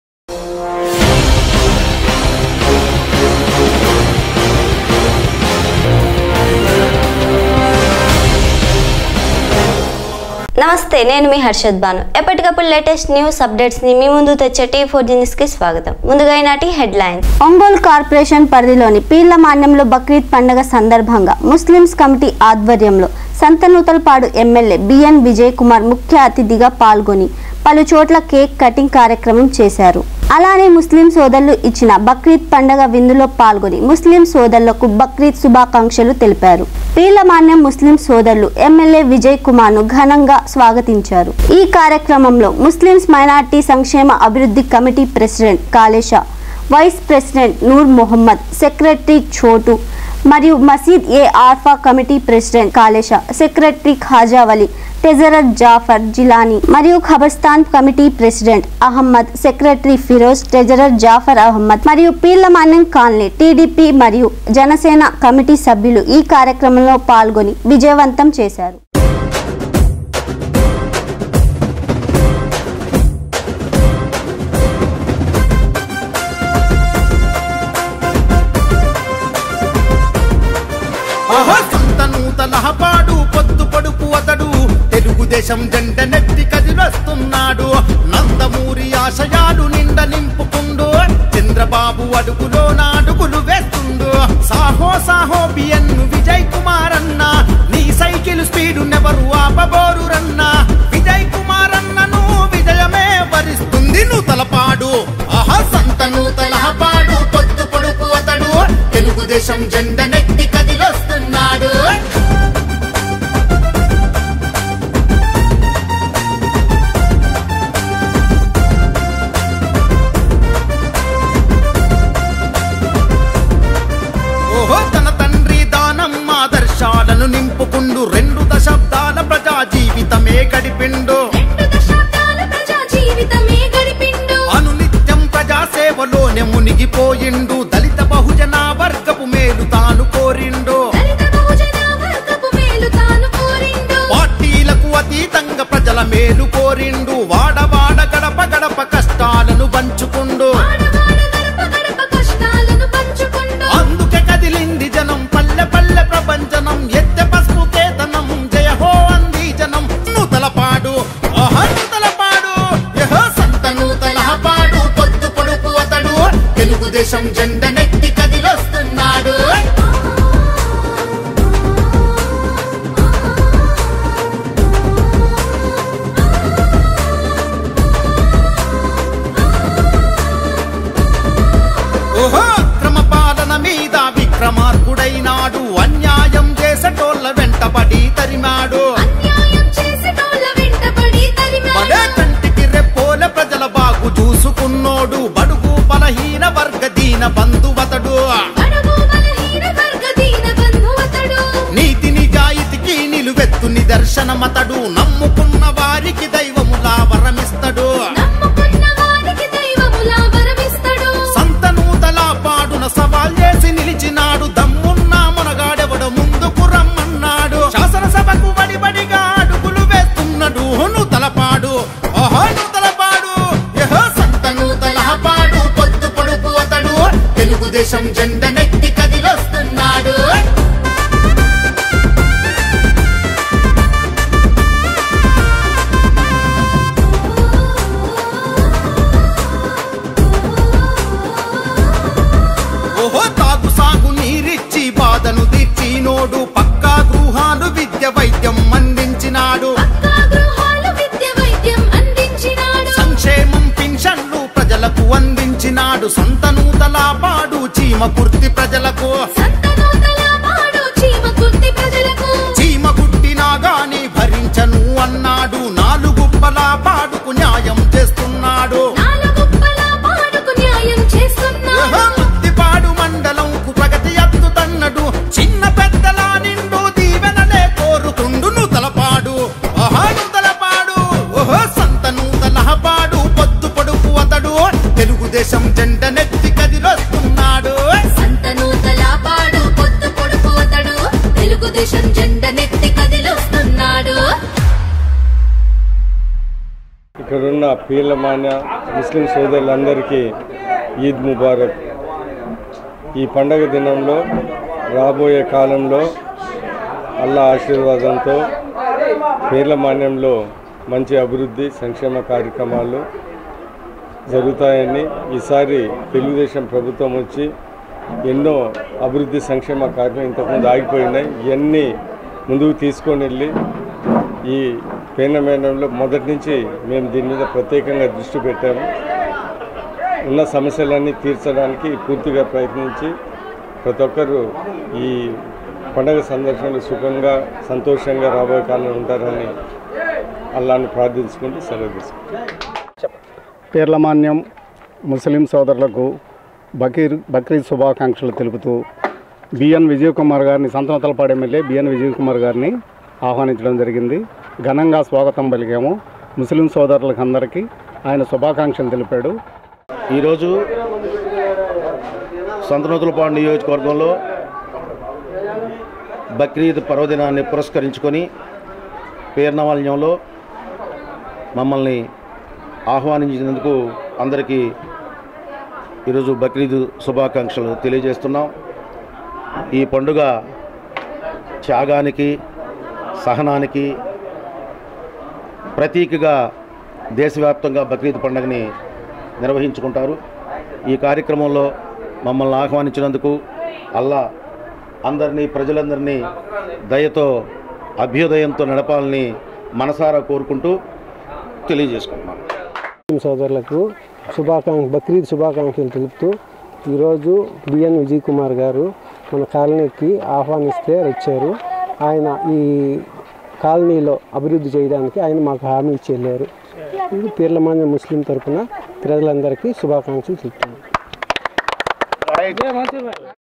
పీర్లమాన్యం బక్రీద్ సందర్భంగా MLA బి.యాన్ విజయ్ కుమార్ కు ముస్లిం సోదరులు ఘన స్వగతం...
పీర్లమాన్యం కమిటీల ఆధ్వర్యంలో బక్రీద్ పండగ వేడుకలు ఘనంగా నిర్వహించారు..
ముఖ్య అతిధిగా పాల్గొన్న MLA విజయ్ కుమార్...